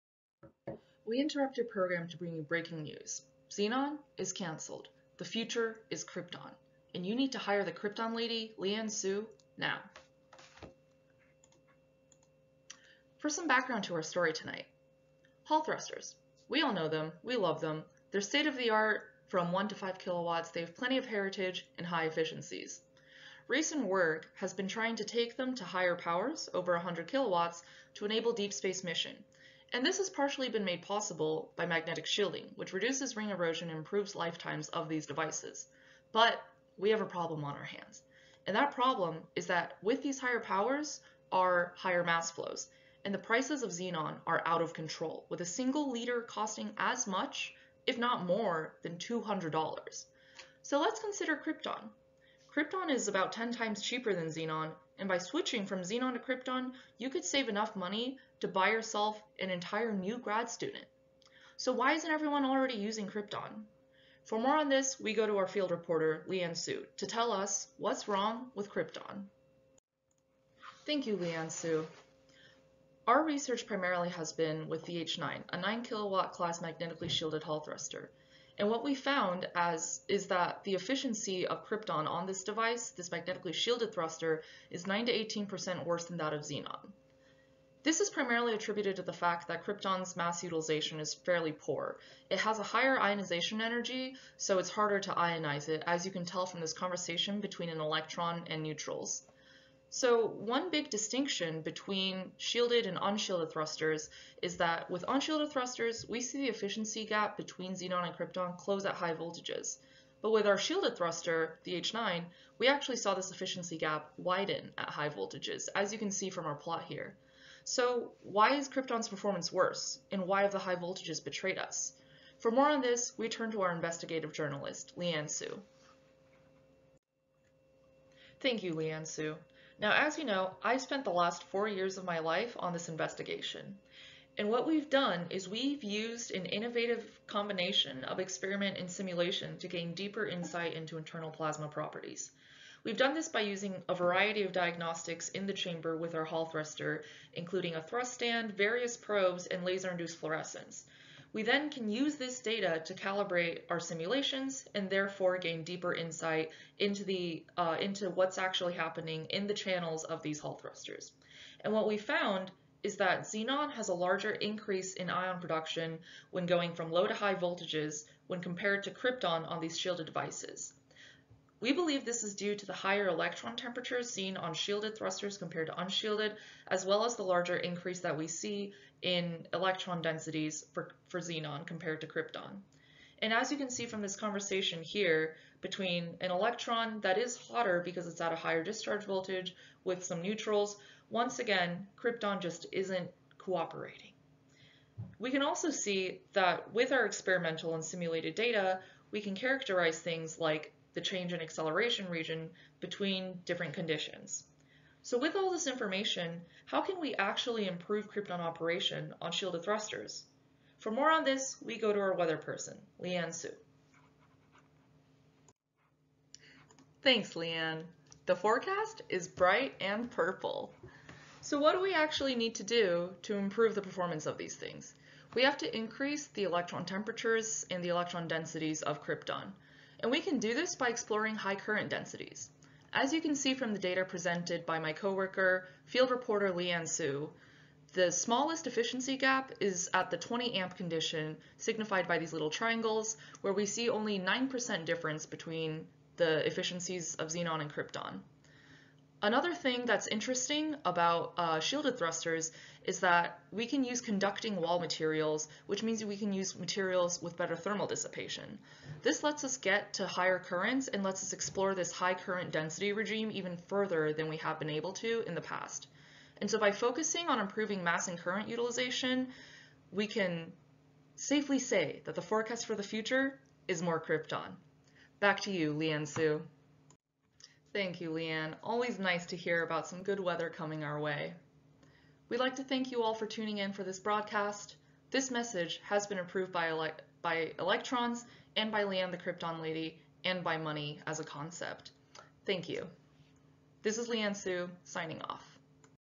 Xenon is CANCELLED, the future is KRYPTON
News anchor Leanne Su, field reporter Leanne Su, investigative journalist Leanne Su, and weatherperson Leanne Su deliver breaking news regarding the future of krypton in the Hall thruster industry.